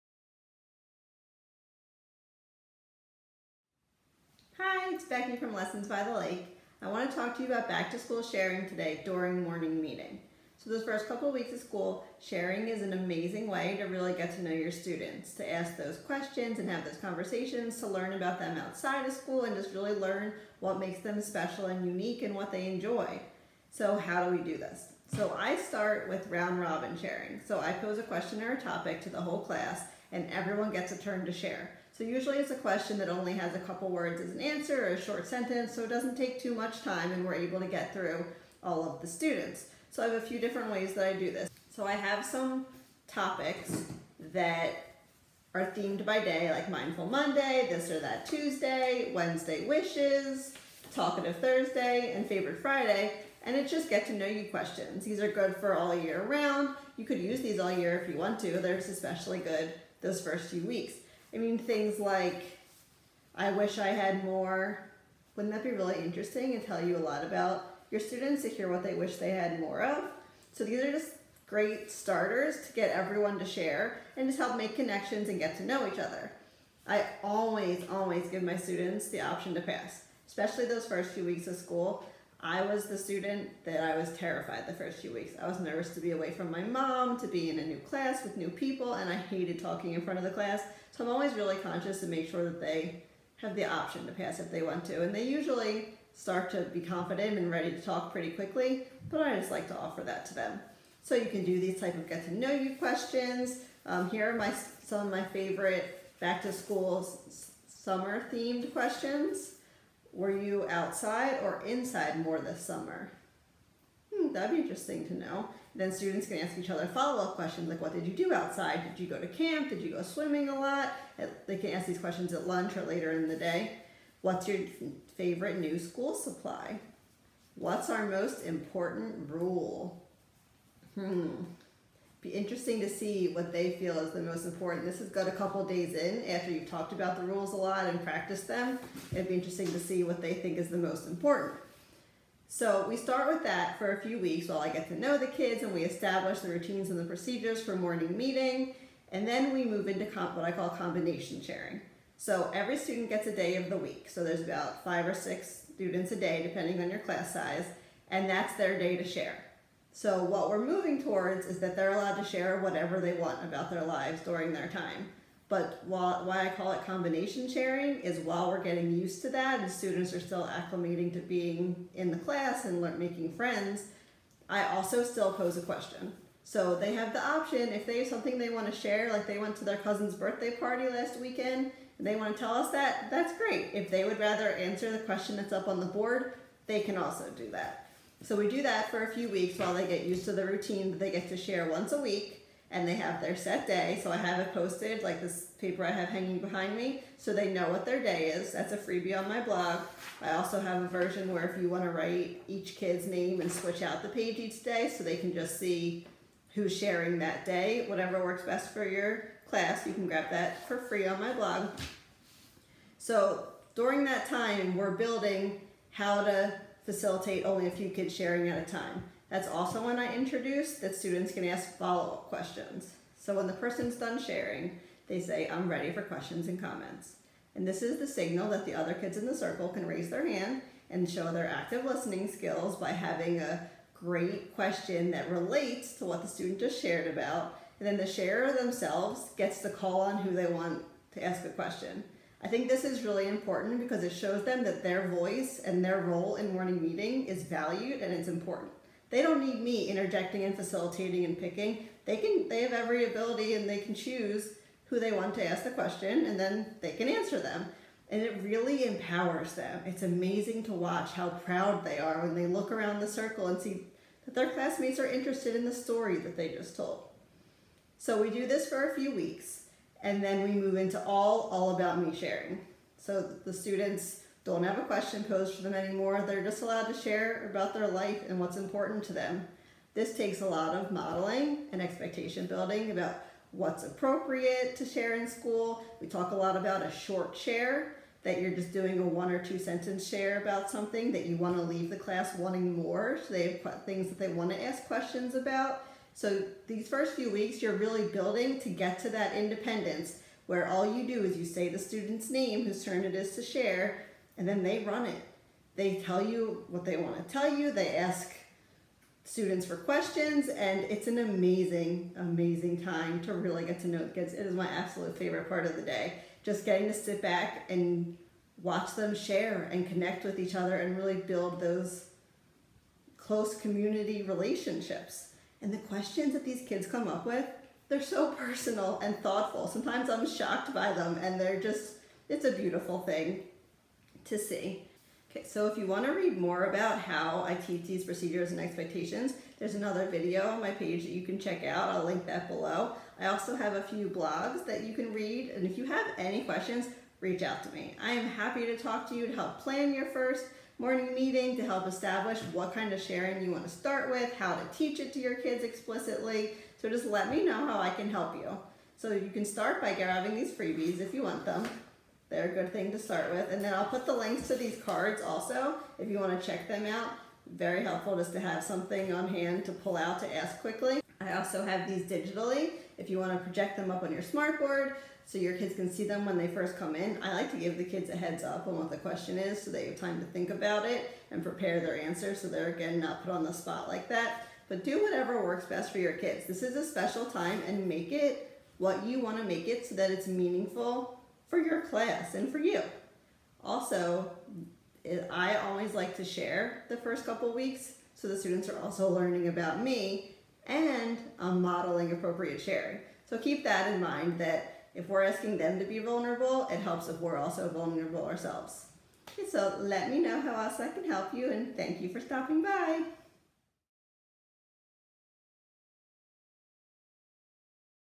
Morning Meeting Back to School Sharing
Let's talk about Morning Meeting Sharing for Back to School! In this video I explain my routine for establishing procedures and expectations for morning meeting sharing. I also share some of my favorite back to school sharing topics.

Links mentioned in the video:
Morning Meeting Sharing Cards (Year Round)
Blog Post- explaining all of the morning meeting sharing options